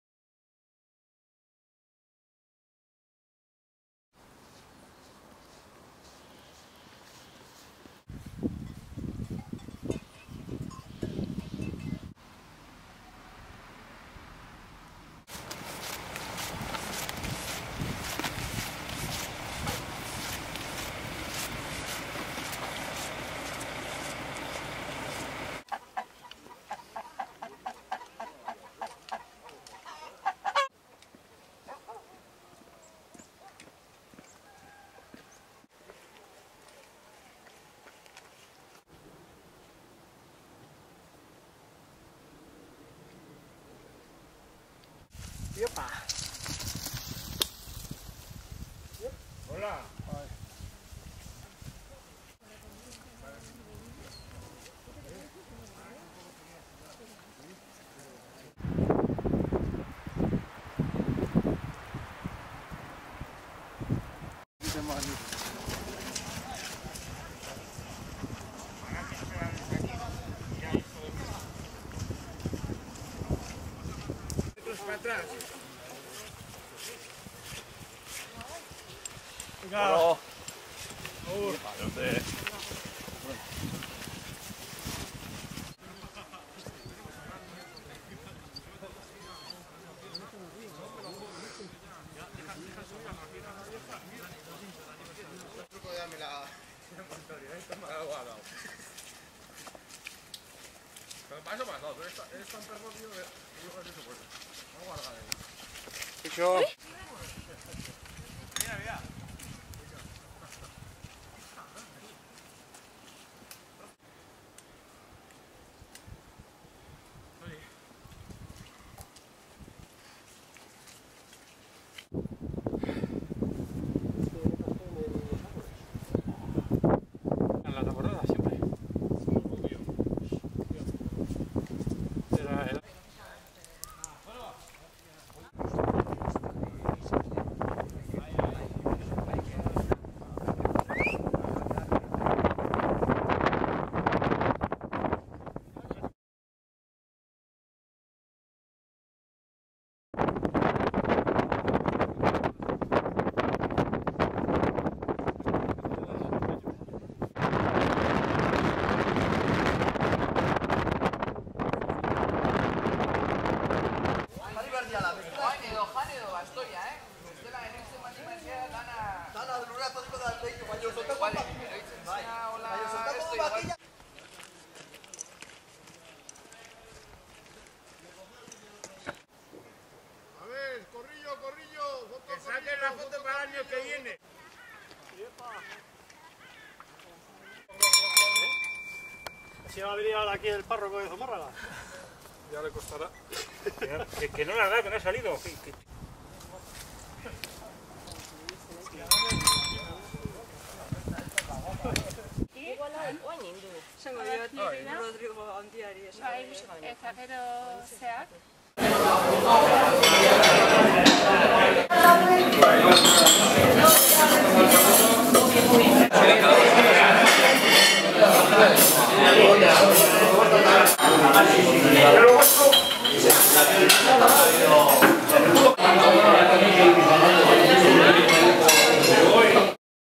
San Silbestre ibilaldia Izazpin eta Belokin. 2011 abendua
San Silbestre mendi ibilaldia Izazpi eta Beloki mendietan, eta Arkaitzeta elkarteko hamarretakoa. 2011 abenduak 31.